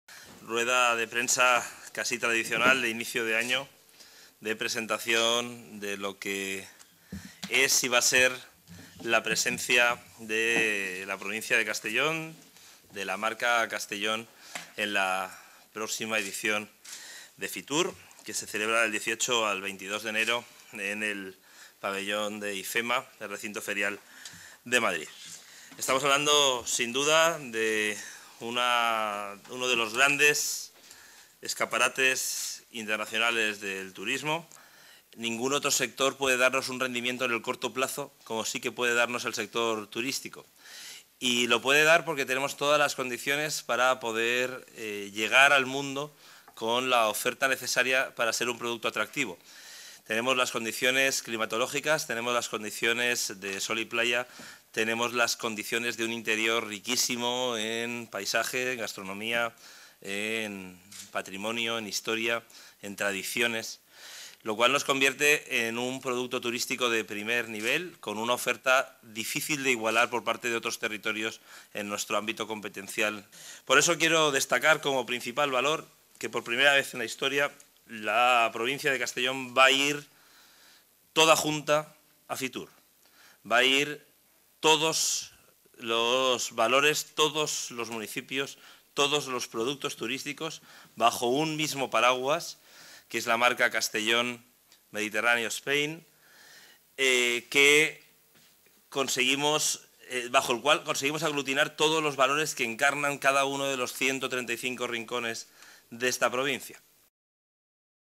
04-01-12 Presentación stand FITUR 2012
Moliner presenta un stand 'de película' que aunará la oferta turística de la provincia en FITUR